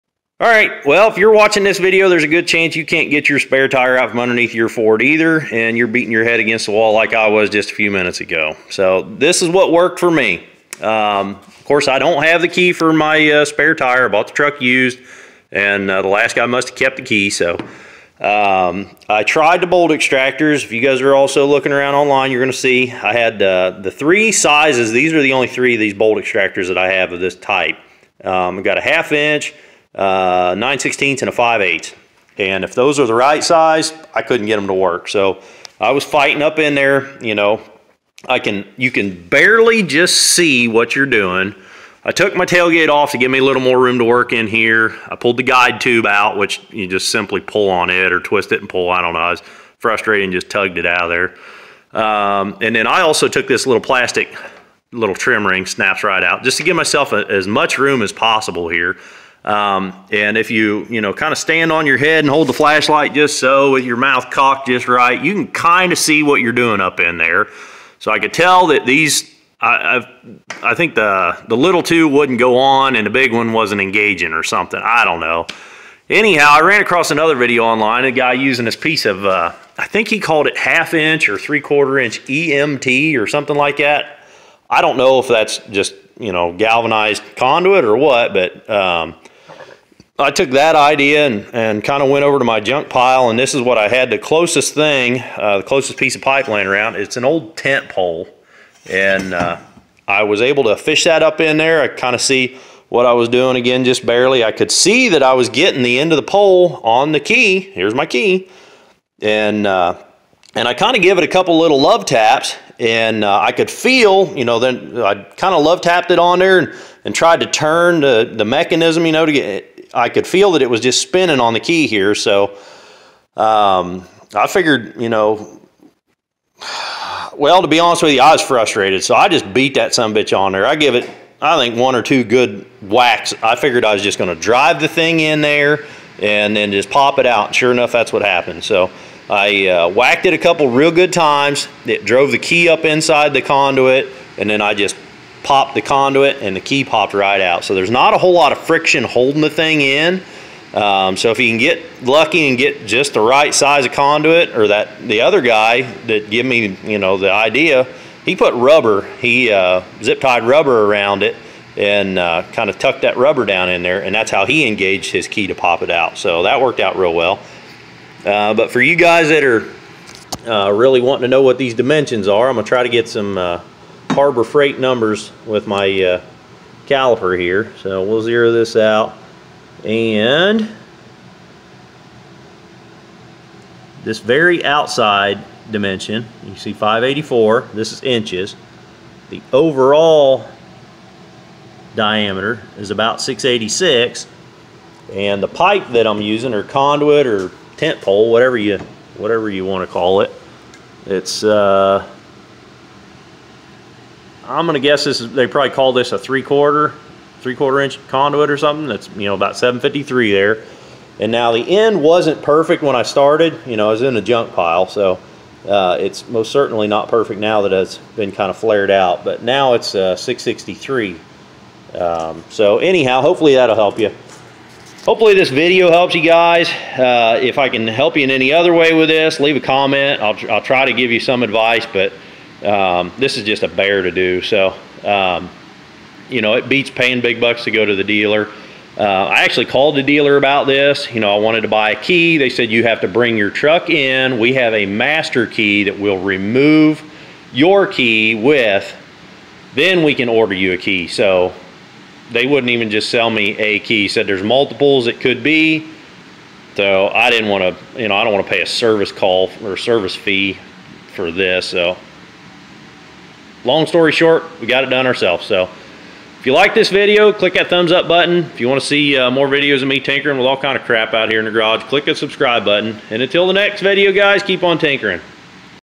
Spare Tire Lock Removal
Removal of the spare tire lock on a 2007 F250.